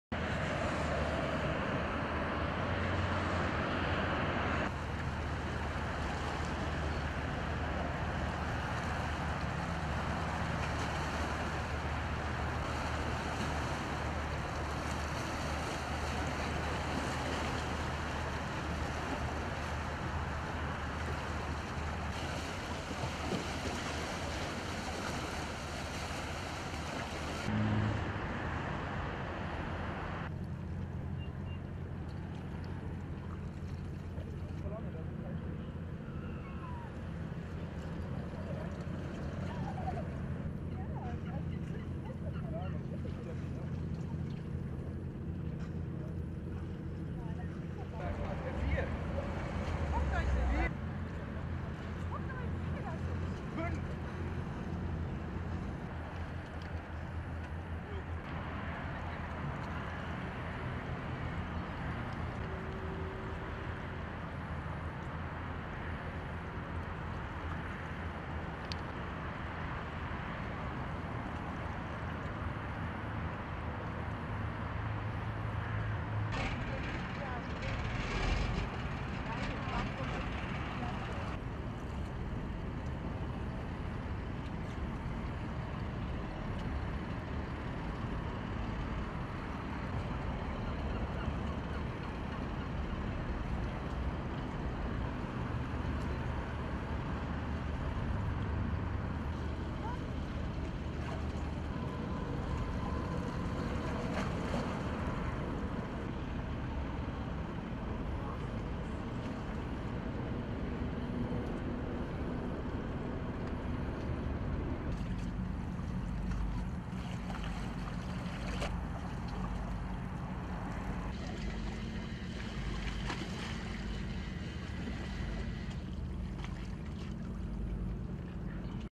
İstanbul'da yunus balıkları görsel şölen oluşturdu
İstanbul'da sabahın erken saatlerinde ortaya çıkan yunus balıkları görsel şölen oluşturdu. Güneş ışıklarıyla parıldayan su yüzeyindeki yunuslar, izleyenleri kendilerine hayran bıraktı.